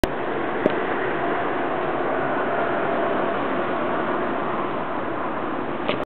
Laurel & Hardy Music Box steps
Bottom of the steps used in the classic Music Box film. Excuse me for filming at night.